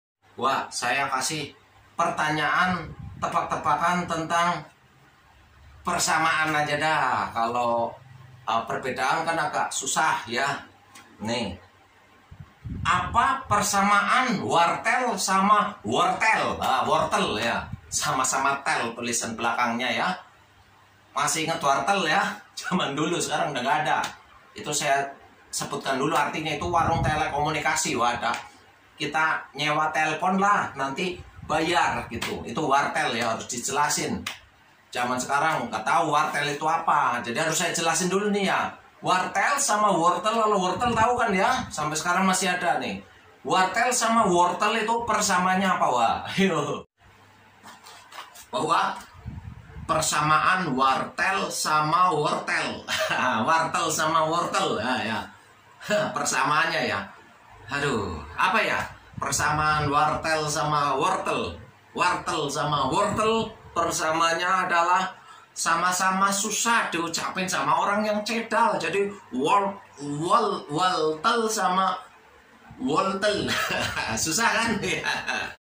Tebak tebakan lucu persamaan wartel sama wortel
Video tebak tebakan lucu persamaan wartel sama wortel. Komedi kocak receh jokes guyon dari @tebeyejokeslaugh semoga ketawa ngakak dan terhibur